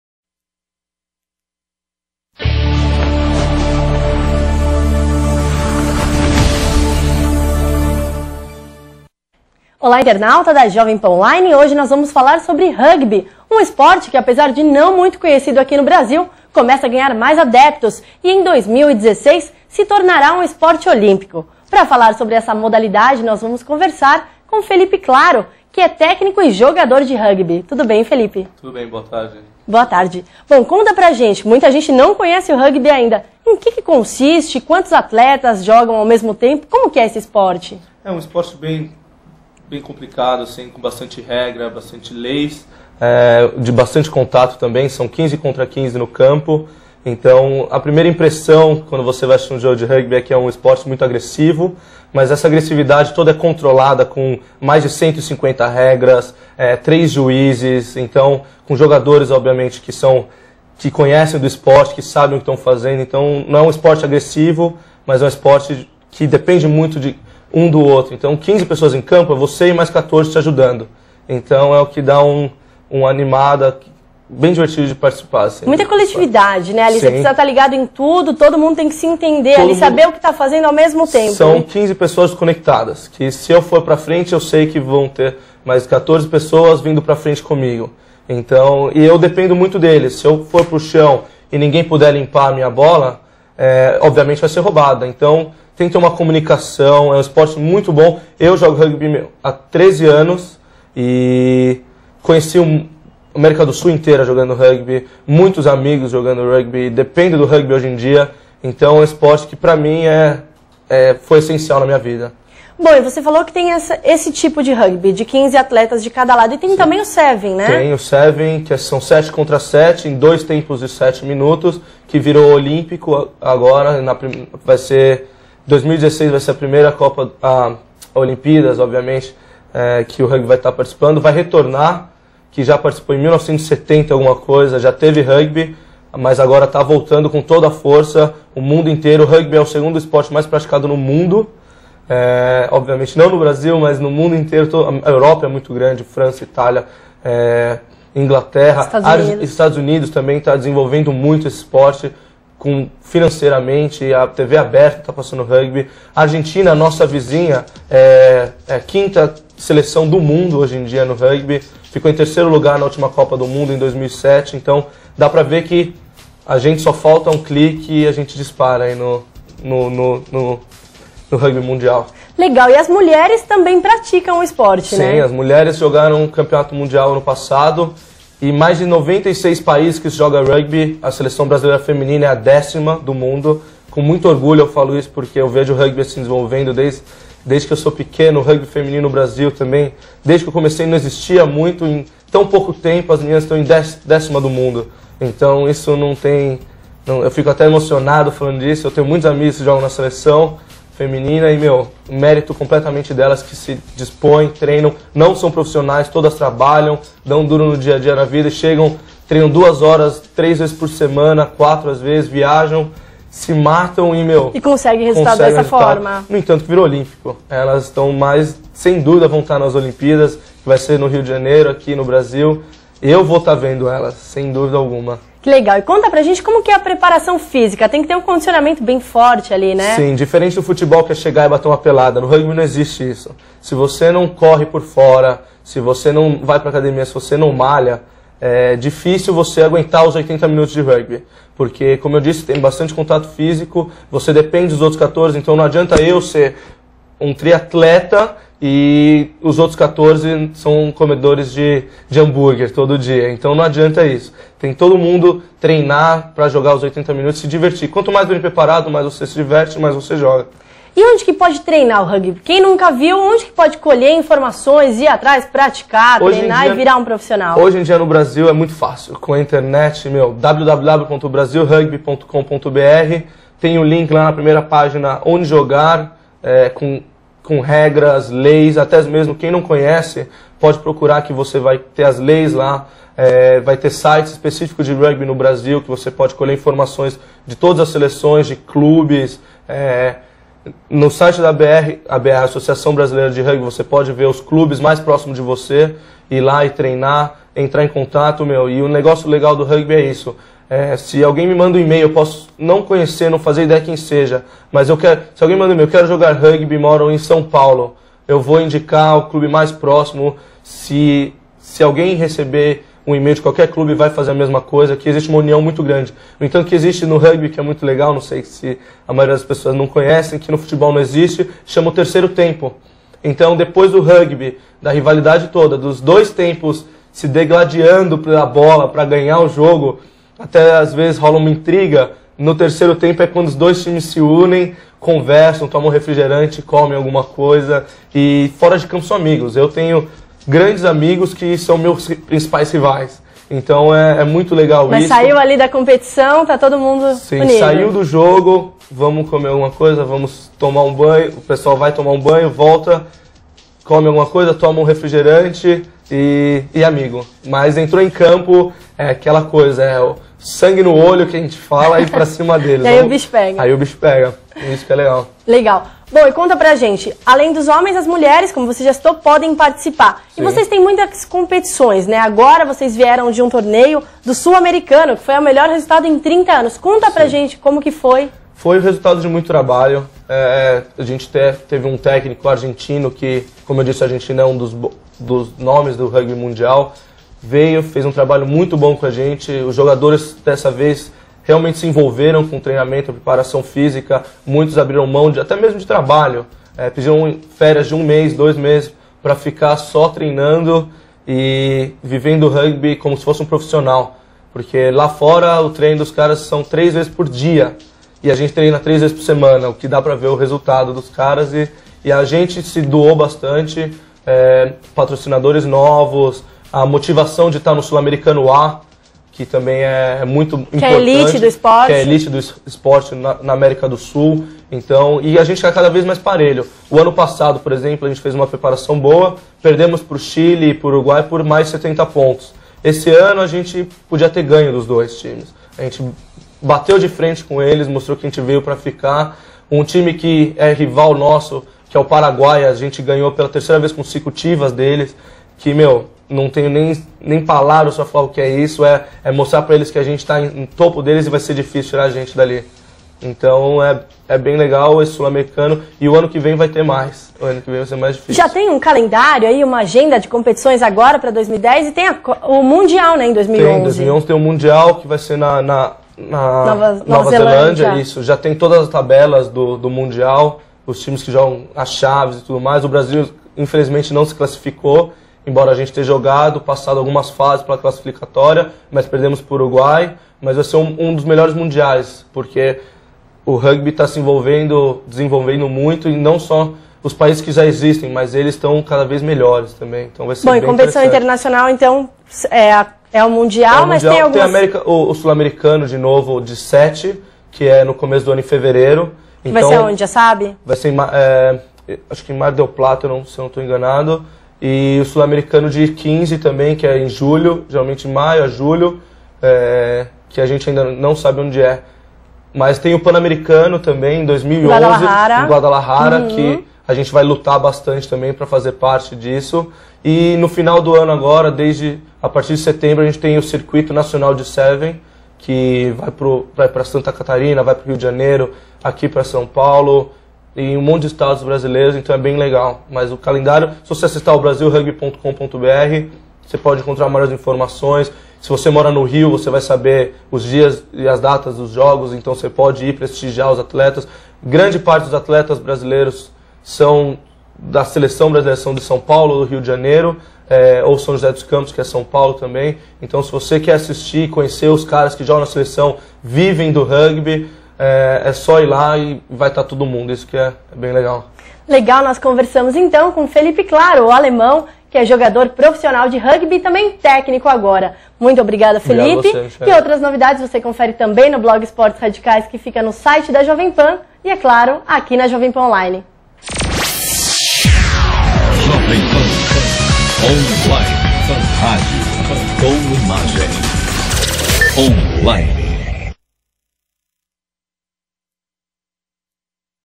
TV Jovem Pan 05 Julho de 2010.wmv
Entrevista de Felipe "Alemão" Claro, atleta da Seleção Brasileira de Rugby que esteve presente no último Sul-Americano da modalidade, à TV Jovem Pan, no último dia 05 de Julho de 2010.